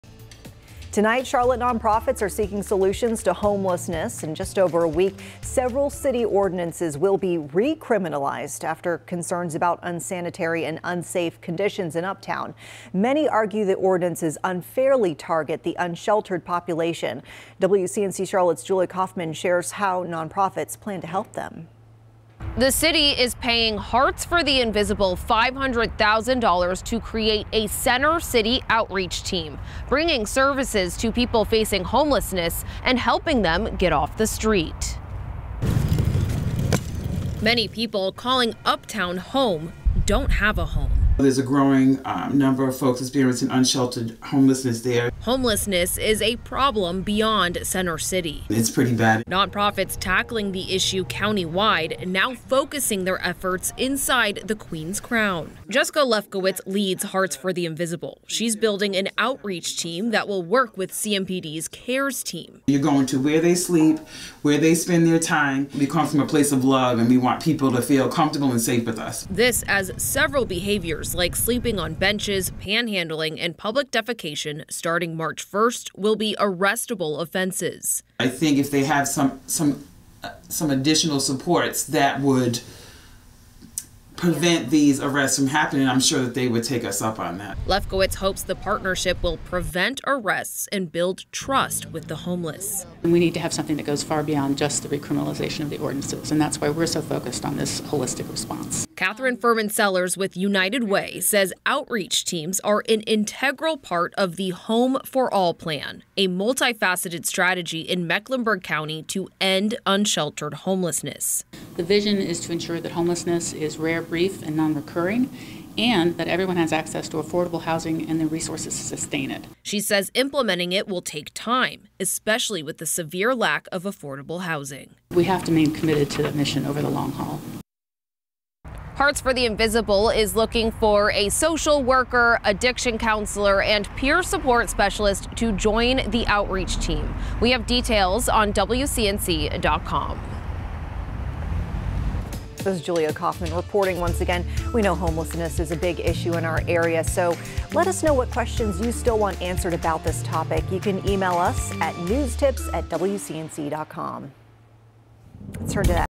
As recriminilization of city ordinances is set to kick in, an outreach team is seeking solutions to
Charlotte nonprofits are seeking solutions to homelessness in Center City.

In just over a week, several city ordinances will be recriminalized after concerns about unsanitary and unsafe conditions in Uptown. Many argue the ordinances unfairly target the unsheltered population. Now, nonprofits are stepping in to help them.

The city is paying Hearts for the Invisible Charlotte Coalition $500,000 to create a Center City outreach team. The nonprofit will bring services to people facing homelessness and help them get off the street.

"There is a growing number of folks experiencing unsheltered homelessness there," Executive
Director for Hearts for the Invisible Jessica Lefkowitz said.